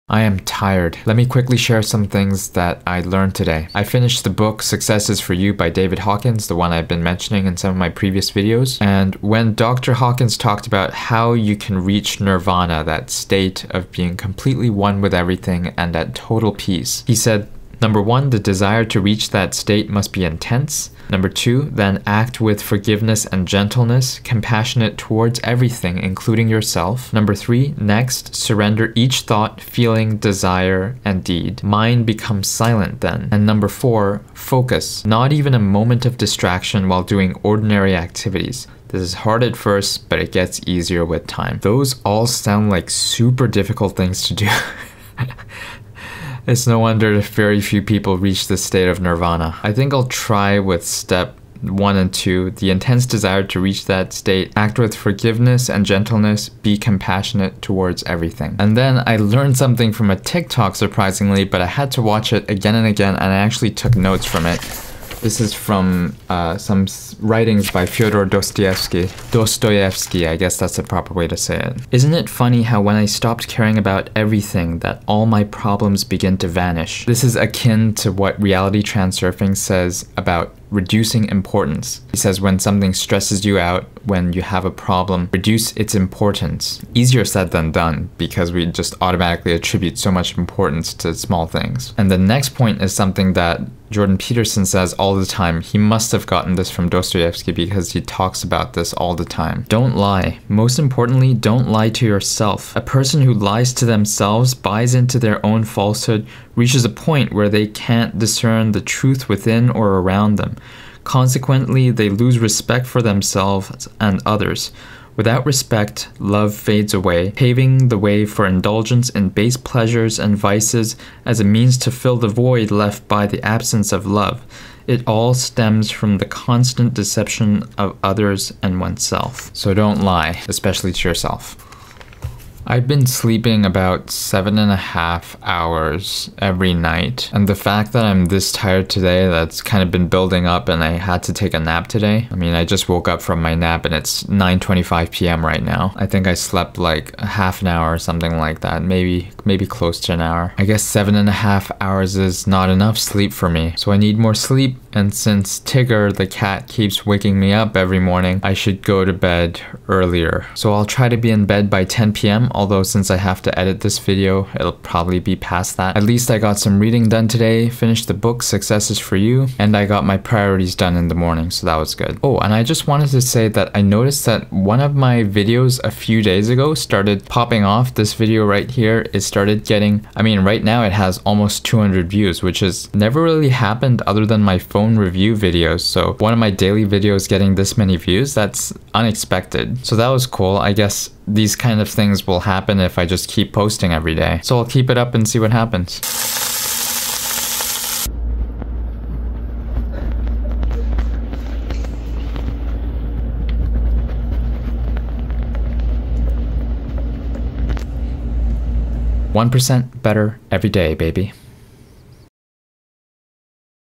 1% Better Day 47 - Stop Caring About Everything
Or rather we should care less about every annoyance.